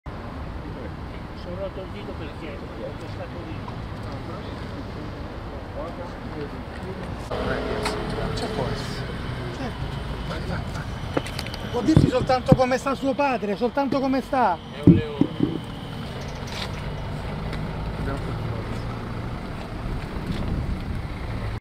Berlusconi, il figlio Pier Silvio esce dal San Raffaele: "Mio padre è un leone"
"É un leone", sono le uniche, ma significative, parole pronunciate da Pier Silvio Berlusconi uscendo dall'ospedale San Raffaele di Milano dove ha fatto visita al padre Silvio per diverse ore nell'arco della giornata. .Di Andrea Lattanzi